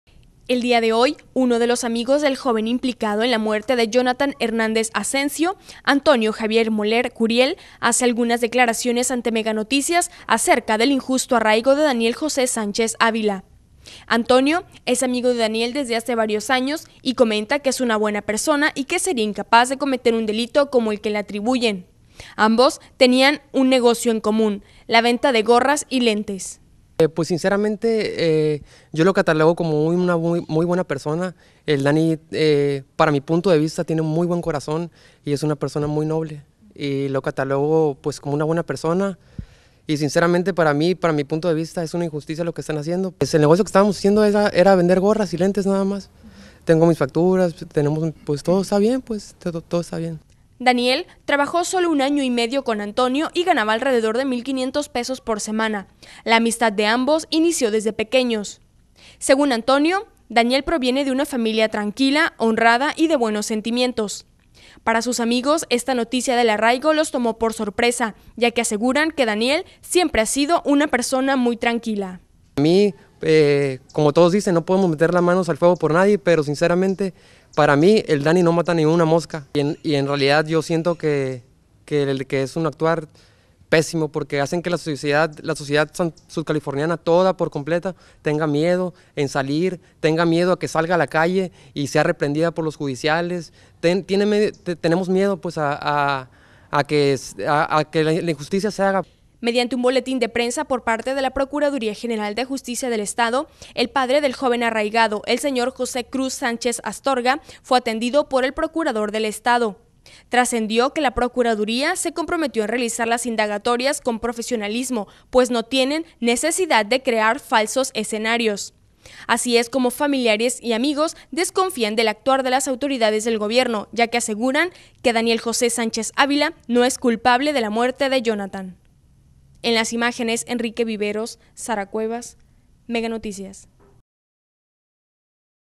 IMPLICADO JONATHAN.mpg
Continúa la desconfianza por parte de amigos de Daniel José Sánchez Ávila arraigado por la muerte de Jonathan, no creen que el sea el asesino.